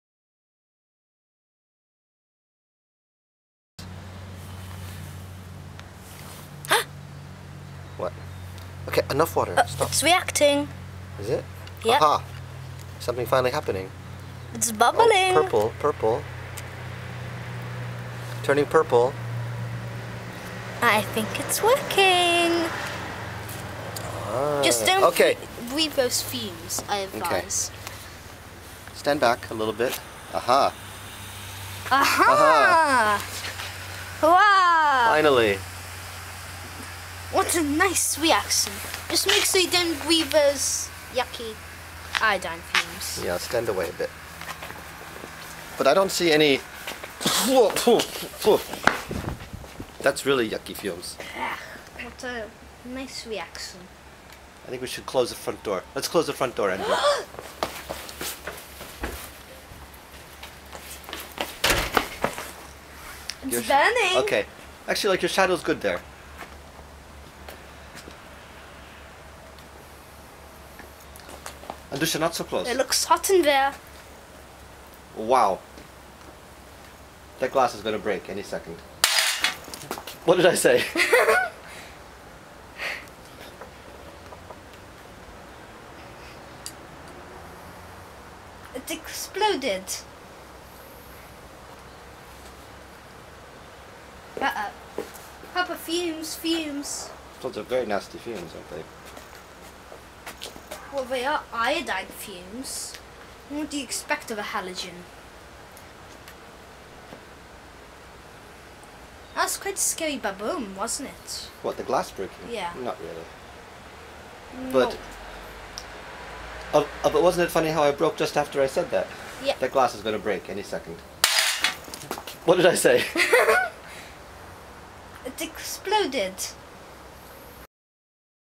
Reaction of Iodine with Aluminium in a Glass Jar
Approximately 1 part of aluminium power was mixed with 2 parts of crushed iodine crystals. A few drops of water were added. This was done in a normal glass jar. This turns out not to be such a good idea since the reaction is very exothermic and so the glass got very hot. The purple vapour is very pretty but definitely avoid inhalling. It feels similar to inhaling chlorine gas.